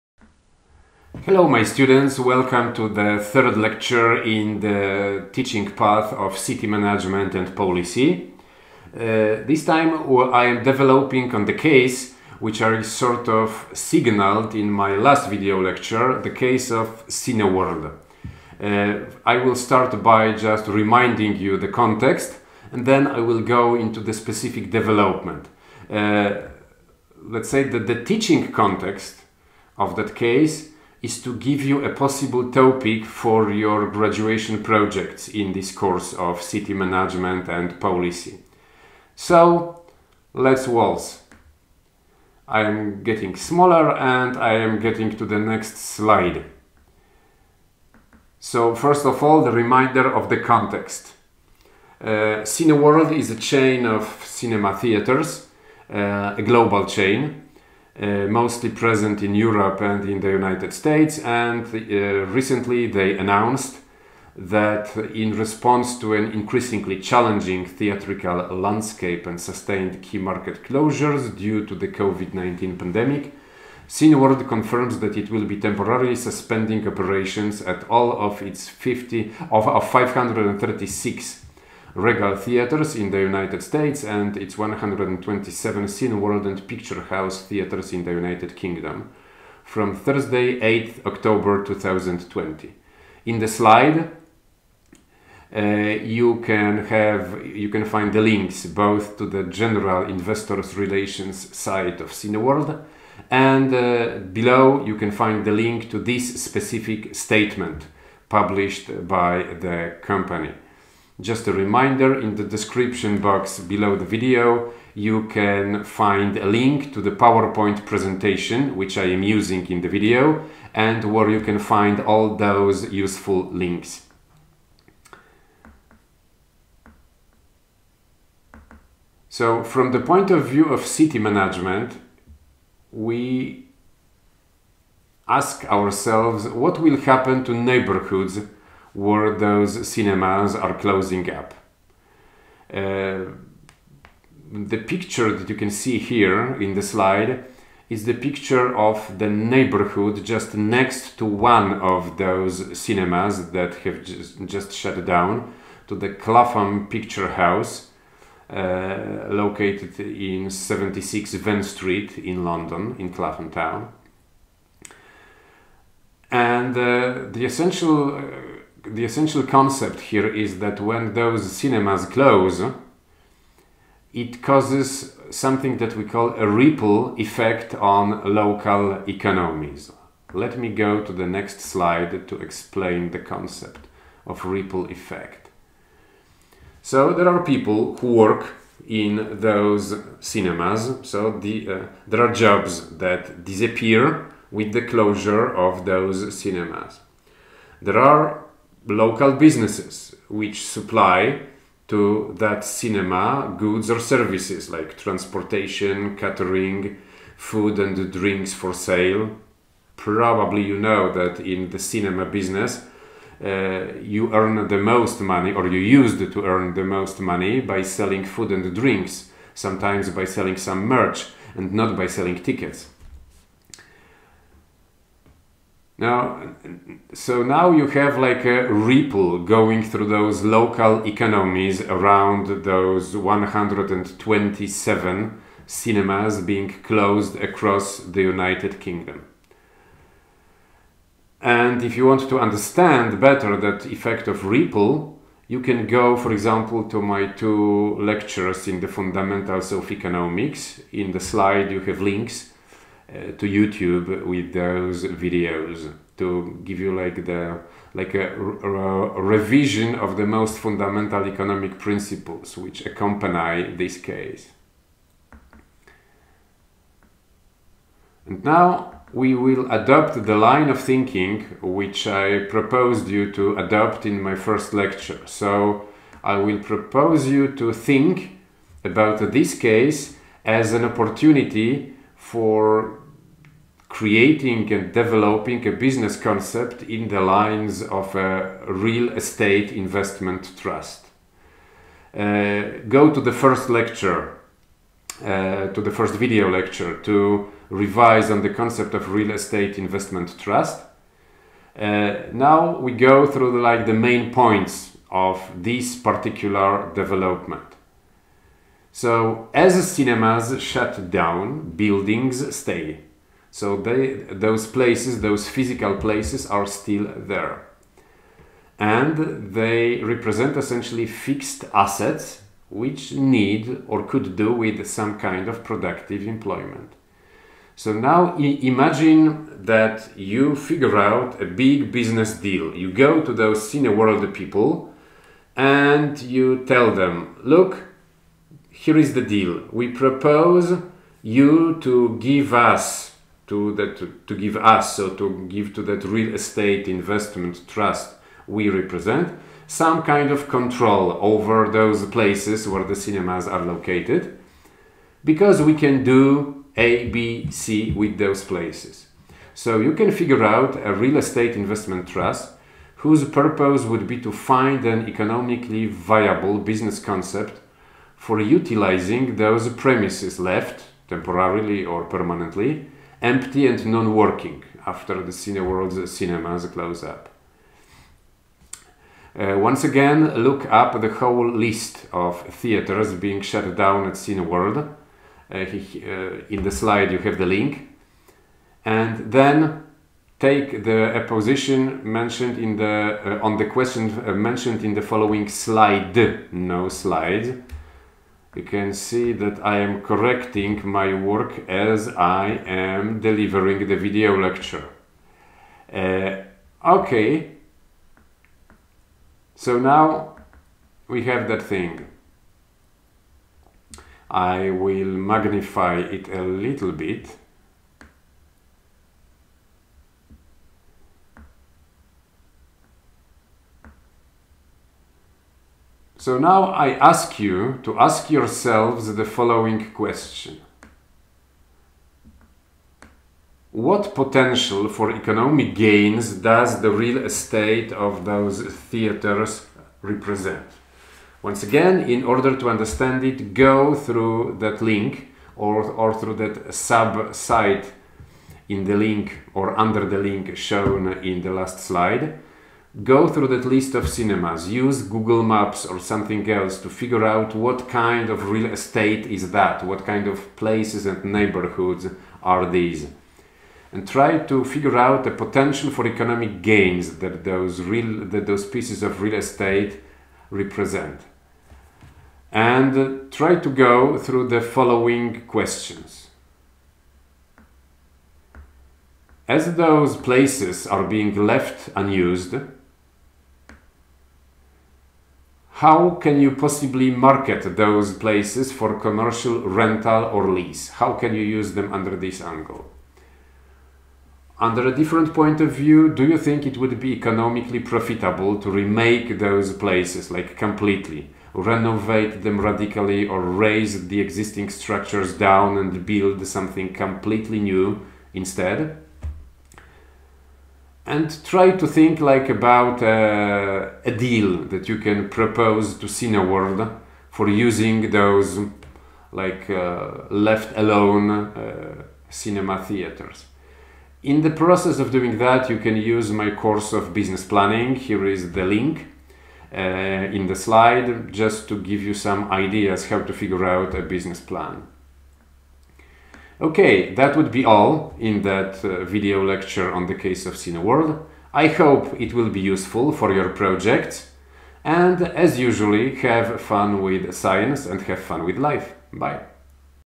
City Management and Policy #3 The case study of Cineworld
I develop on the case mentioned in the opening lecture of the course, namely on the case of Cineworld closing hundreds of their cinema theatres. I want you, my students, to think about opportunities in urban development that it creates. You can download the corresponding power point presentation